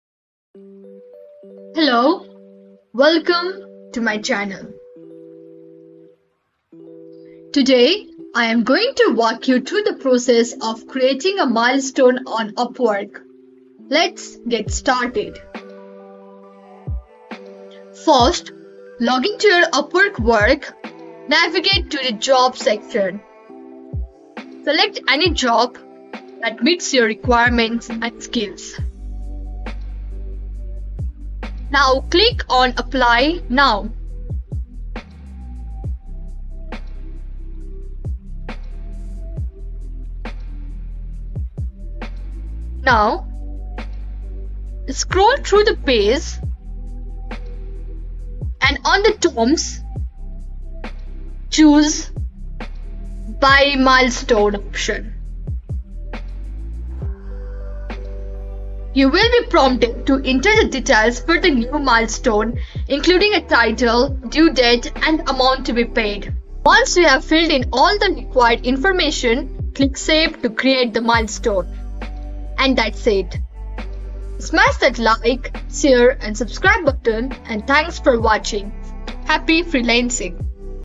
How to Create Milestone on Upwork (2024) | Register Milestone on Upwork
Discover how to create milestones on Upwork with our comprehensive tutorial. Break down your projects into manageable parts and ensure clear communication with clients. We’ll guide you through the process of setting and managing milestones for successful project completion.